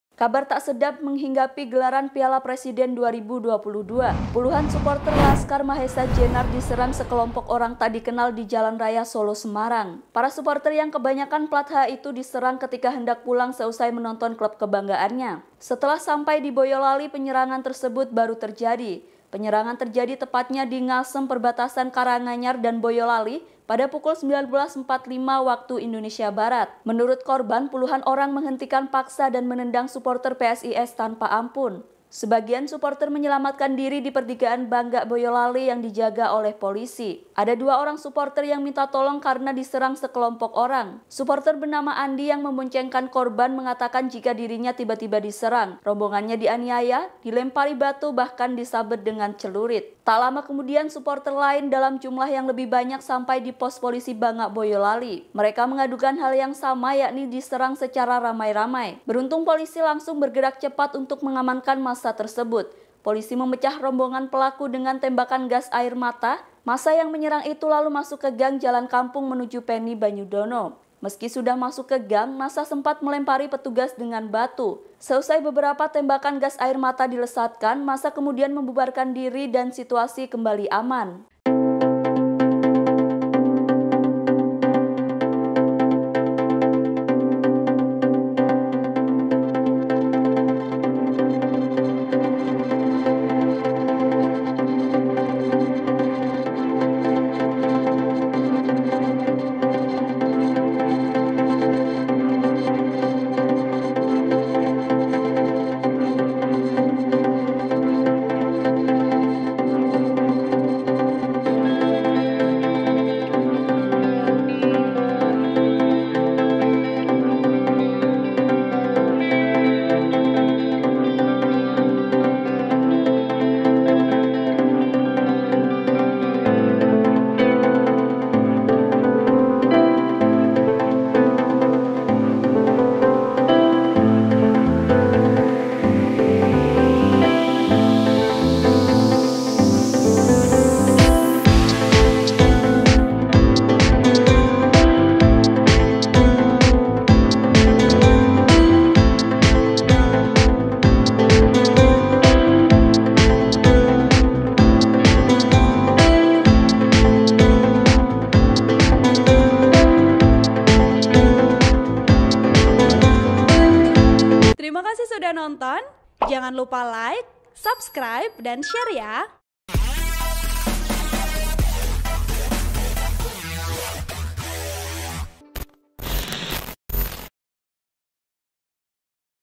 Rombongan Suporter PSIS Diserang, Dianiaya hingga Kena Sabetan Celurit
Kabar tak sedap menghinggapi gelaran Piala Presiden 2022. Puluhan suporter Laskar Mahesa Jenar diserang sekelompok orang tak dikenal di Jalan Raya Solo-Semarang.
Para suporter yang kebanyakan plat H itu diserang ketika hendak pulang seusai menonton klub kebanggannya.
Setelah sampainya di Boyolali, penyerangan tersebut baru terjadi.
Penyerangan terjadi tepatnya di Ngasem, perbatasan Karanganyar dan Boyolali pada pukul 19.45 WIB.
Menurut korban, puluhan orang menghentikan paksa dan menendang suporter PSIS tanpa ampun.
Sebagian suporter menyelamatkan diri di pertigaan Bangak, Boyolali yang dijaga oleh polisi.
Ada dua orang suporter yang minta tolong karena diserang sekelompok orang.
Suporter bernama Andi yang memboncengkan korban mengatakan jika dirinya tiba-tiba diserang.
Rombongannya dianiaya, dilempari batu bahkan disabet dengan celurit. Tak lama kemudian, suporter lain dalam jumlah yang lebih banyak sampai juga di Pos Polisi Bangak Boyolali.
Mereka mengadukan hal yang sama, yakni diserang ramai-ramai. Beruntung polisi langsung bergerak cepat untuk mengamankan masa tersebut.
 Polisi memecah rombongan pelaku dengan tembakan gas air mata.
Masa yang menyerang itu lalu masuk ke gang jalan kampung menuju Peni, Banyudono.
Meski sudah masuk ke gang, masa sempat melempari petugas dengan batu.
Seusai beberapa tembakan gas air mata dilesatkan, masa kemudian membubarkan diri dan situasi kembali aman.